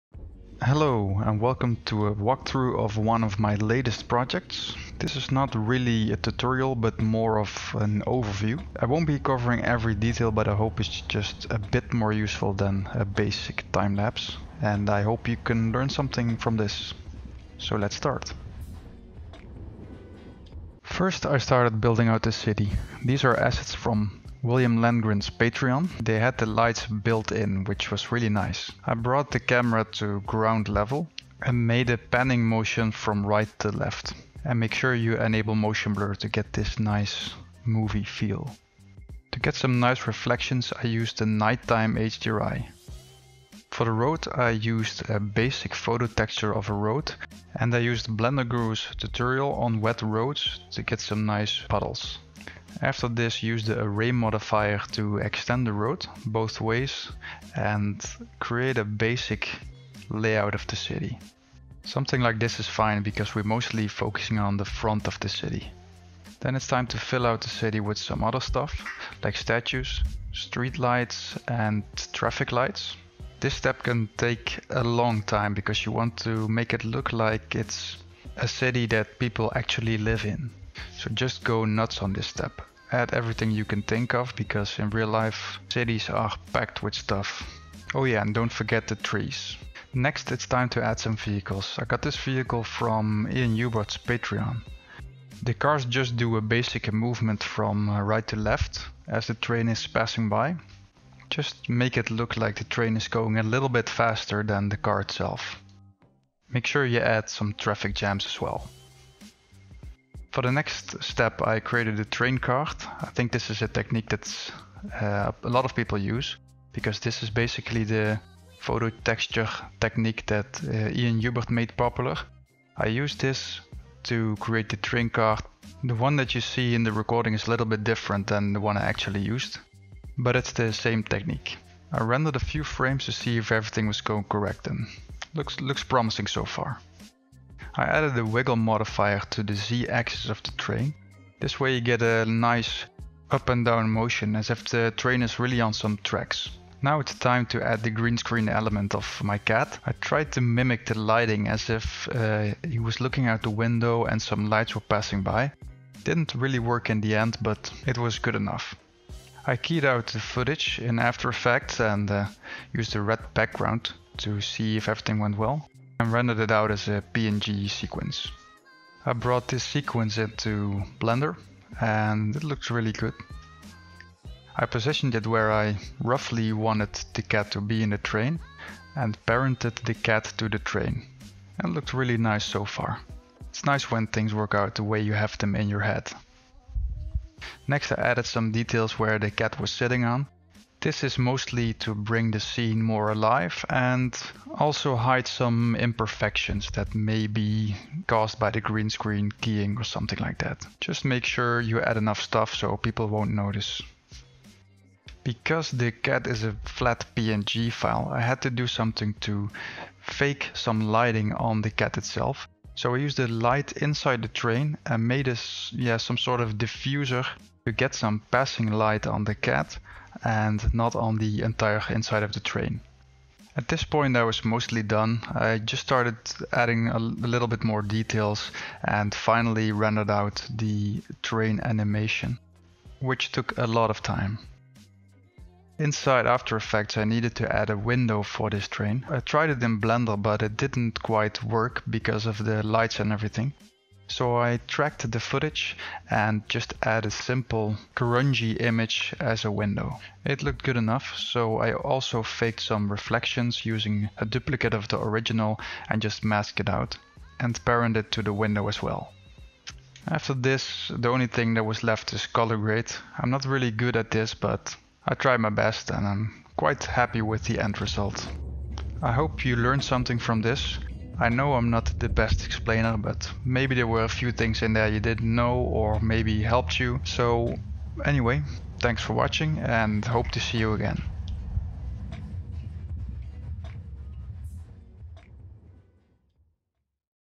Adding Green Screen Elements in Blender - Workflow
This is my first overview of a project.
Train ride.
Using Green screen elements in blender.

Its kinda to learn myself some presentation skills and maybe help other by giving some insight in my blender workflow.